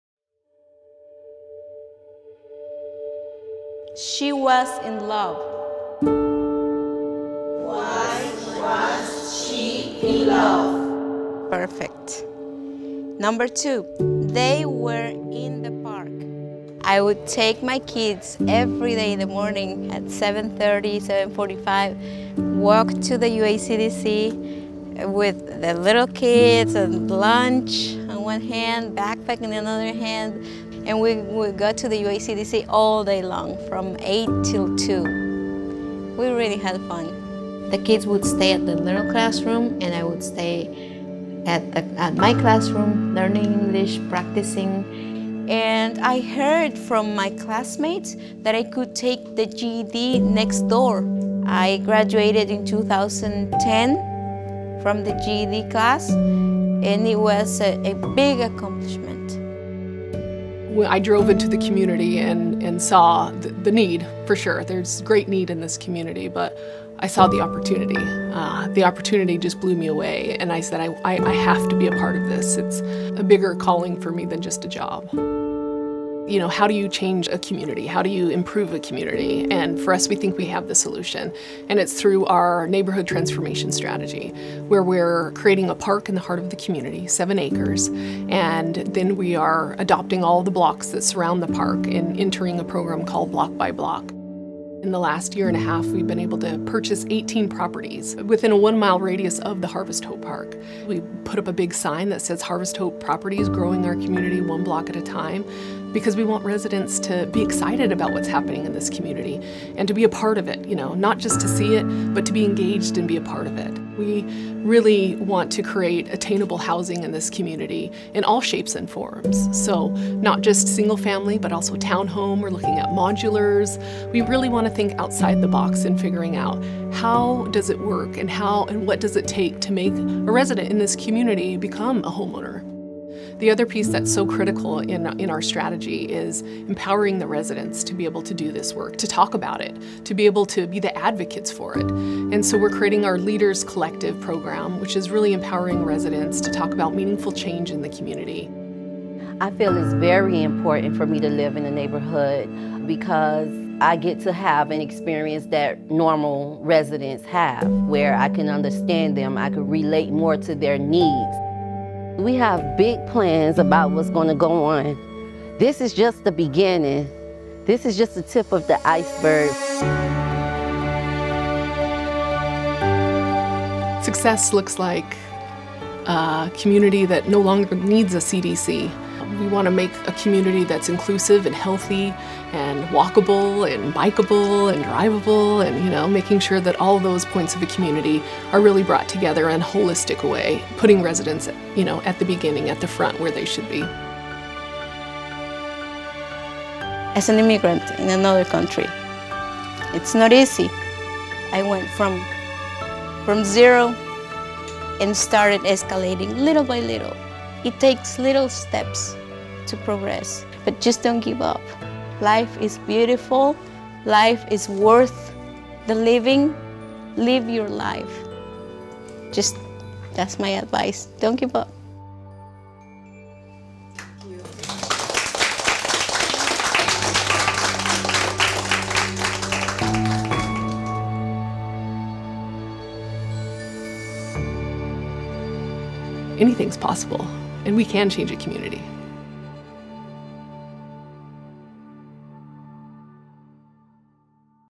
University Area CDC Transformation Strategy Video
An overview of University Area CDC's Transformation Strategy for the University Area community.